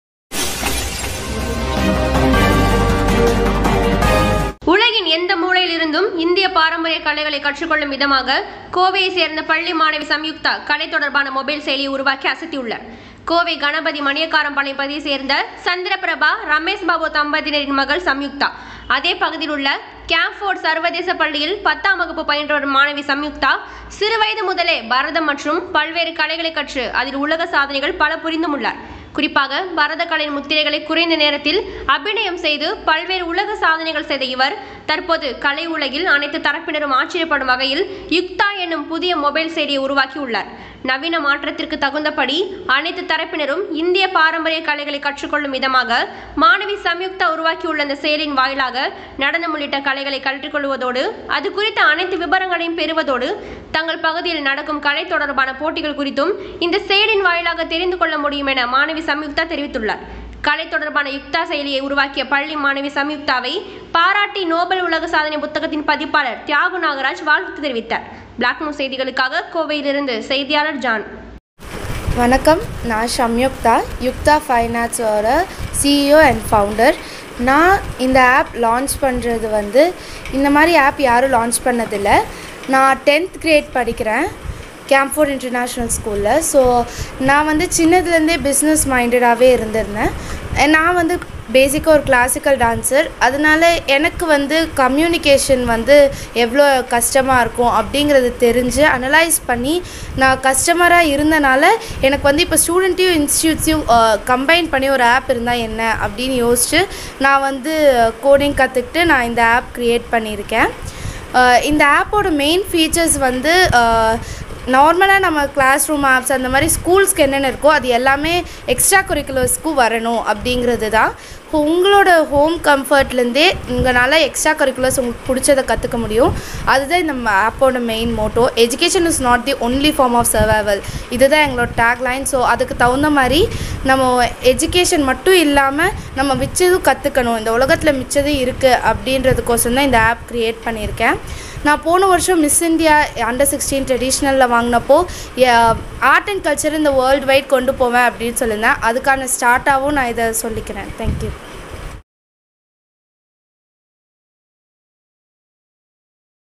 பள்ளி மாணவி சம்யுத்தா கலை தொடர்பான மொபைல் செயலியை உருவாக்கிய அசத்தியுள்ளார்
உலகின் எந்த மூலையில் இருந்தும் இந்திய பாரம்பரிய கலைகளை கற்றுக்கொள்ளும் விதமாக கோவையை சேர்ந்த பள்ளி மாணவி சம்யுக்தா  கலை தொடர்பான மொபைல் செயலியை உருவாக்கி அசத்தியுள்ளார்..

கோவை கணபதி மணியகாரம்பாளையம் பகுதியை சேர்ந்த சந்திரபிரபா,ரமேஷ் பாபு தம்பதியினரின் மகள் சம்யுக்தா.அதே பகுதியில் உள்ள கேம்ஃபோர்டு சர்வதேச பள்ளியில் பத்தாம் வகுப்பு பயின்று வரும் மாணவி சம்யுக்தா சிறு வயது முதலே பரதம் மற்றும் பல்வேறு கலைகளை கற்று அதில் உலக சாதனைகள் பல புரிந்தும் உள்ளார்.குறிப்பாக பரத கலையின் முத்திரைகளை குறைந்த நேரத்தில் அபிநயம் செய்து பல்வேறு உலக சாரனைகள் செய்த இவர்,தற்போது கலை உலகில் அனைத்து தரப்பினரும் ஆச்சரியப்படும் வகையில் யுக்தா எனும் புதிய மொபைல் செயலியை உருவாக்கி உள்ளார்.நவீன மாற்றத்திற்கு தகுந்த படி அனைத்து தரப்பினரும் இந்திய பாரம்பரிய கலைகளை கற்றும் கொள்ளும் விதமாக மாணவி சம்யுக்தா உருவாக்கி உள்ள இந்த செயலியின் வாயிலாக நடனம் உள்ளிட்ட கலைகளை கற்றுக்கொள்வதோடு அது குறித்த அனைத்து விபரங்களையும் பெறுவதோடு, தங்கள் பகுதியில் நடக்கும் கலை தொடர்பான போட்டிகள் குறித்தும் இந்த செயலியின் வாயிலாக தெரிந்து கொள்ள முடியும் என மாணவி சம்யுக்தா தெரிவித்துள்ளார்..கலை தொடர்பான யுக்தா செயலியை உருவாக்கிய பள்ளி மாணவி சம்யுக்தாவை பாராட்டி நோபள் உலக சாதனை புத்தகத்தின் பதிப்பாளர் தியாகு நாகராஜ் வாழ்த்து தெரிவித்தார்..